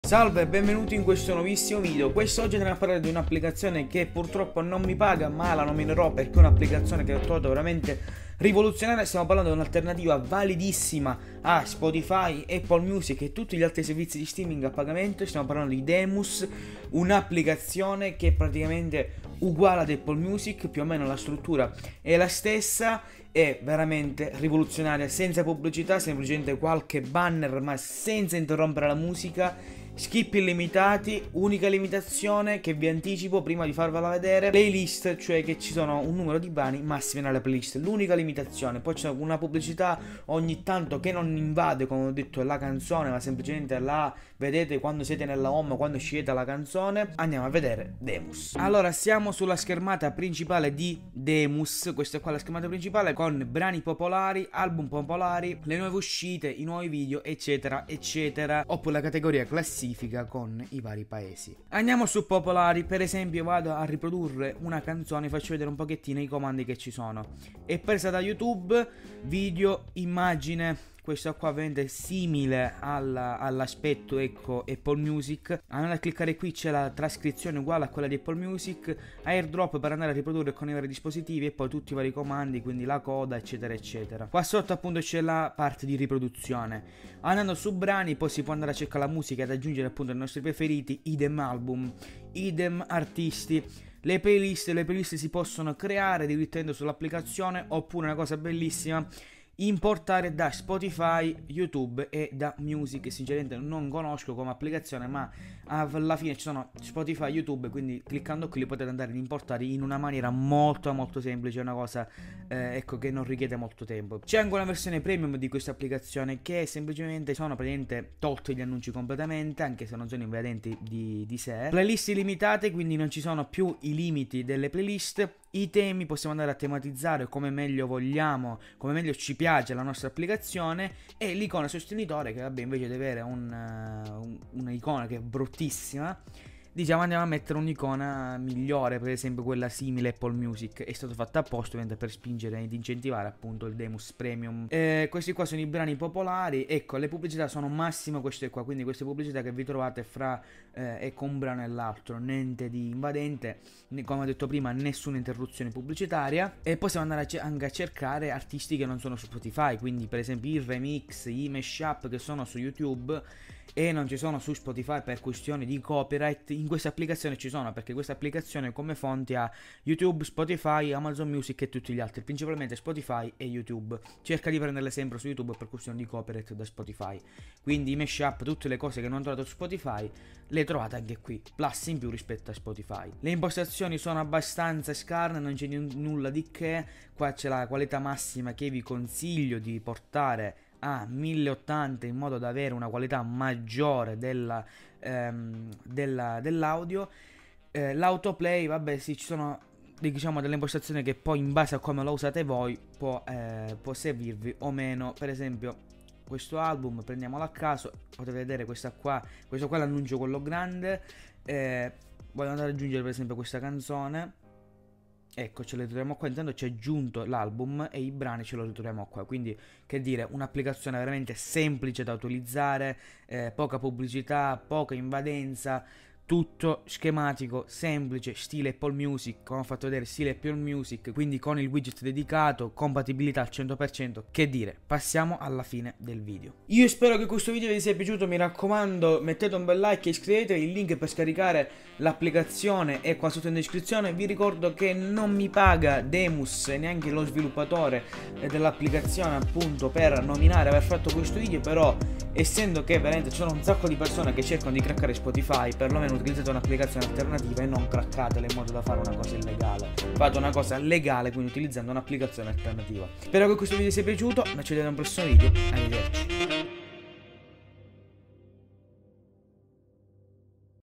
Addio Spotify! DEMUS: MUSICA GRATIS e SENZA PUBBLICITÀ su iPhone!
Demus è un'app che permette di ascoltare musica in streaming gratuitamente e senza interruzioni pubblicitarie durante la riproduzione. Disponibile su Apple Store, offre un'interfaccia intuitiva, la possibilità di creare playlist e l'importazione di brani da Spotify, YouTube e altre piattaforme. La versione Premium, con un pagamento unico, rimuove le pubblicità e sblocca playlist illimitate.

Demus rappresenta una valida alternativa a Spotify per chi cerca un'opzione gratuita per ascoltare musica online. *Questo video non ha finalità promozionali.*

Lorenzo Perucci
LP PRODUCTIONS